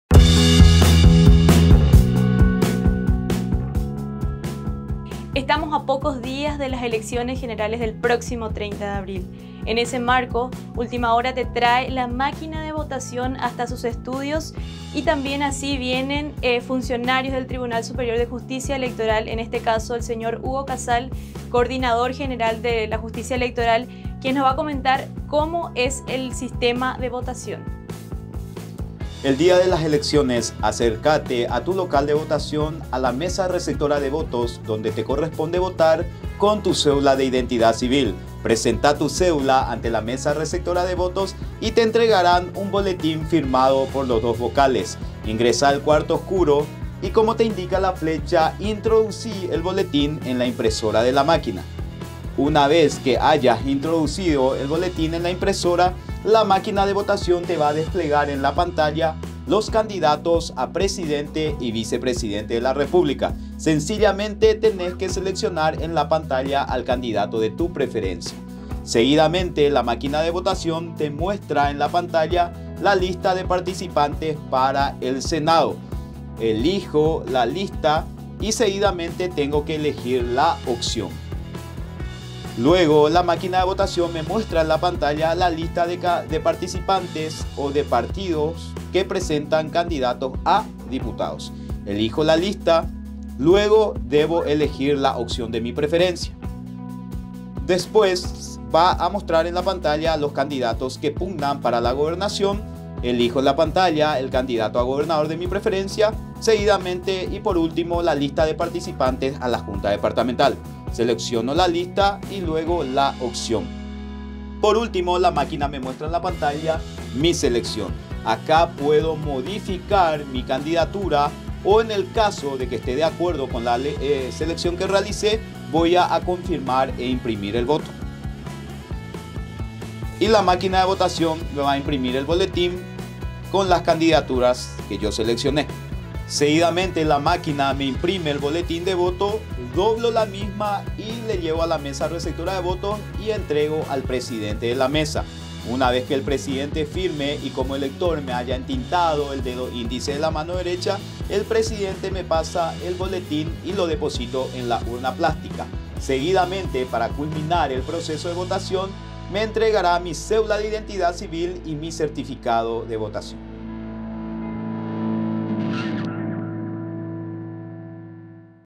Paso a paso: Así se usa la máquina de votación - ÚH
Hugo Cazal, coordinador general de Recursos Electorales del Tribunal Superior de Justicia Electoral (TSJE), mostró de manera detallada el funcionamiento de la máquina de votación y explicó cómo la ciudadanía podrá sufragar el próximo 30 de abril.